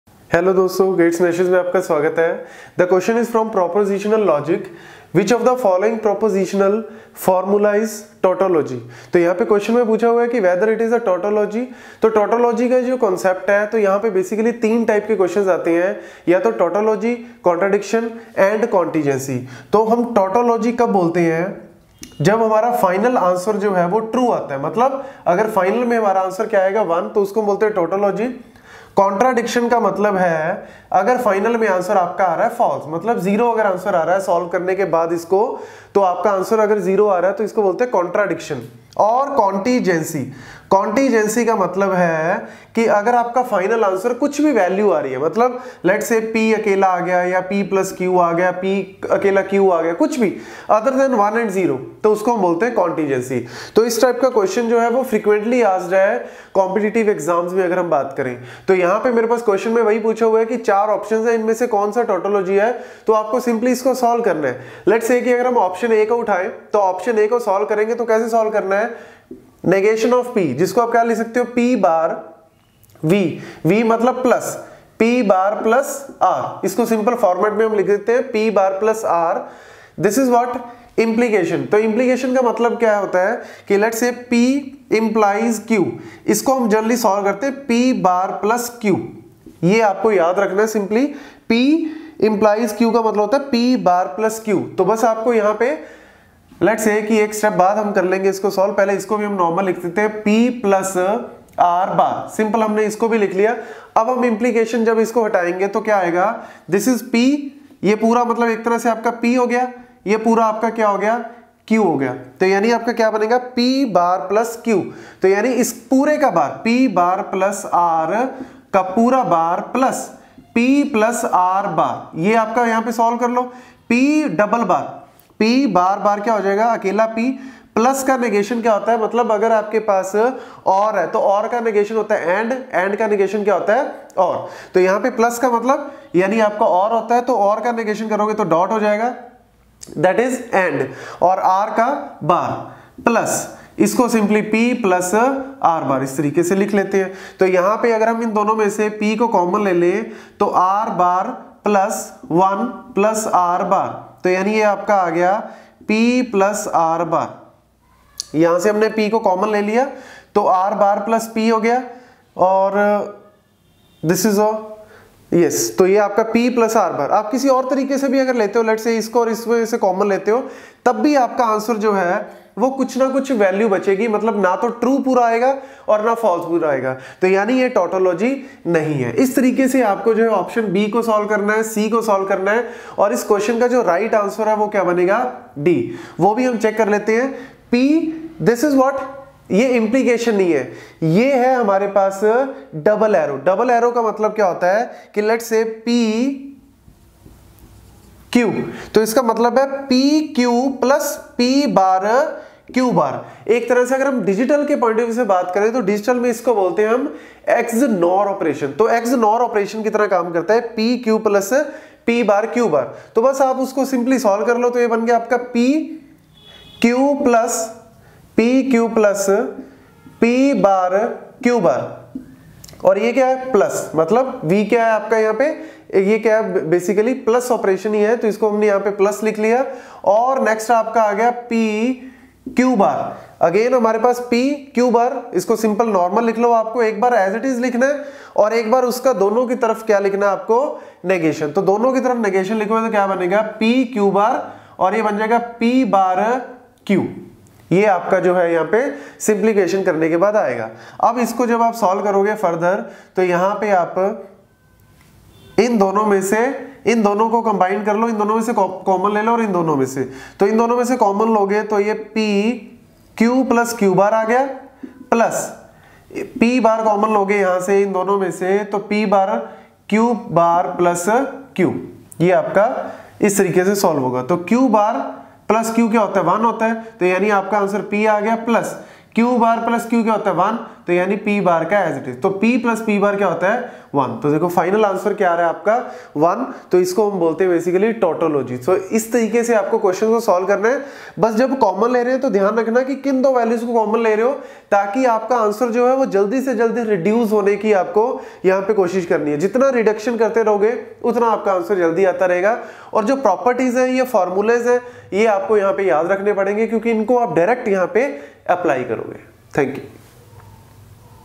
Question on Mathematical Logic | Discrete Maths | UGC NTA NET May/June 2021
According to Week#1 day#1 schedule, Questions are given and students have shown the tremendous response. So based on your Polling we have selected two questions (1 and 4) which got the highest weightage so in this video explanation of question no 1 is given. Check it out and Be ready for tomorrow questions as per schedule.